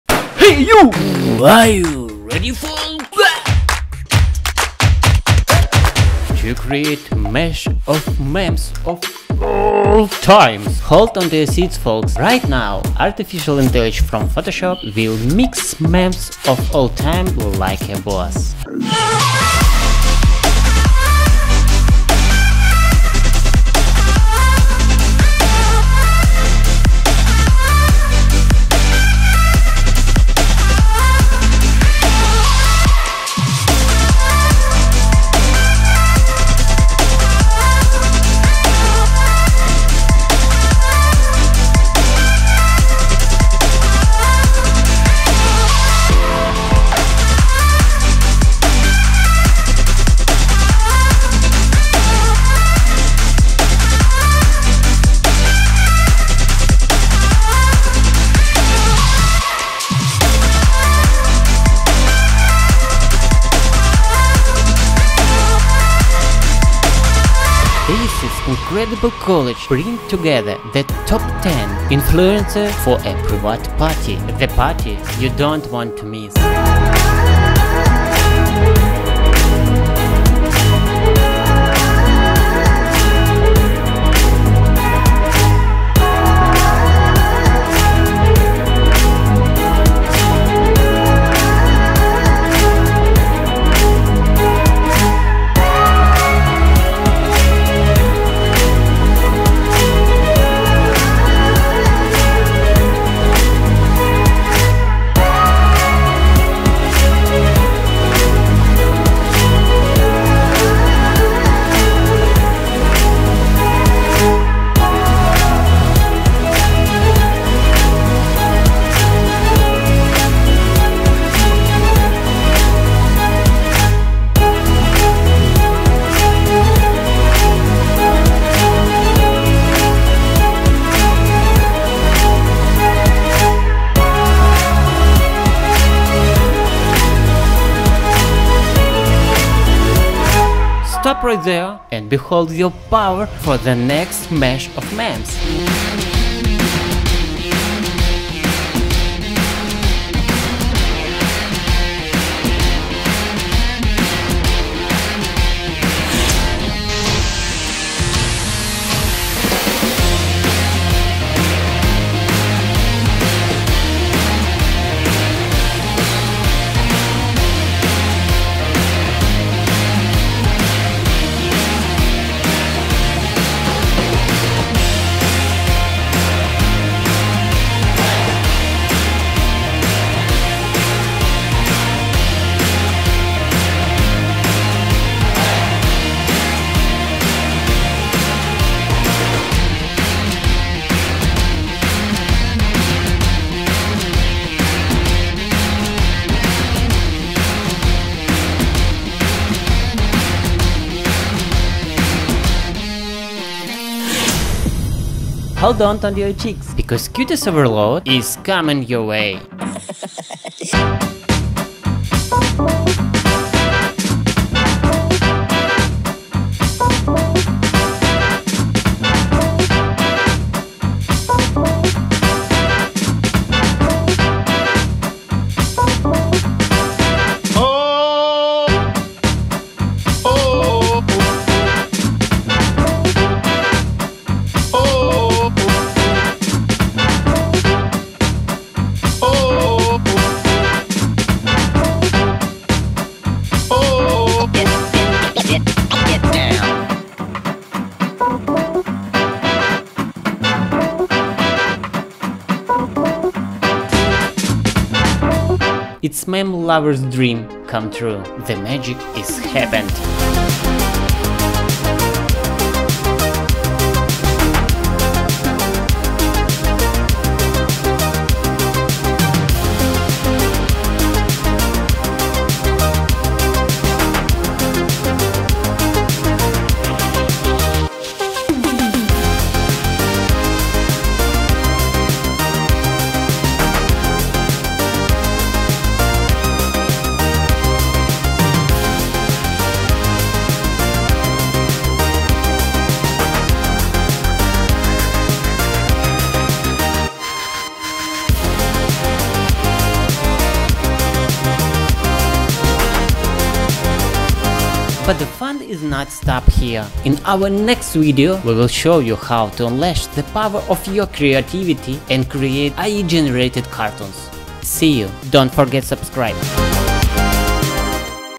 Adobe Firefly : Meme Epic Collab You Never Saw Coming! 🔥
Get ready to unlock the full power of your imagination with @Synthetic-Media 🚀In this mind-bending video, we bring you an extraordinary journey into the world of memes like you've never seen before💥
Using the incredible Adobe Firefly AI , we've created an epic collage that merges the top memes from across the internet into one mind-blowing masterpiece. 🎨
Prepare to be captivated as legendary characters, grumpy babies, superhero alliances, and more collide in a visual extravaganza.
😱 This is the ultimate meme mashup you never saw coming!
Join us on this exhilarating ride and prepare to have your funny bone tickled, your mind expanded, and your laughter ignited. 😄
Don't miss out on this unforgettable adventure with Synthetic Media! 🔥

Timecodes:
00:00 Powerfull Intro
00:18 Meme Legends Unite: The Ultimate Table of Laughter
01:07 Golden Celebrity Bash: Top 10 TikTok Bloggers Unite
02:20 Superheroes Mix: Super Sequel from Marvel, DC, Harry Potter, Avatar
03:24 Meme Support Squad: Thumbs Up to Robert Downey Jr.'s Wisdom
04:15 The Grumpy Baby and the Sassy Fox
04:37 The Power Trio That Demands Attention
05:03 Watch More, Outro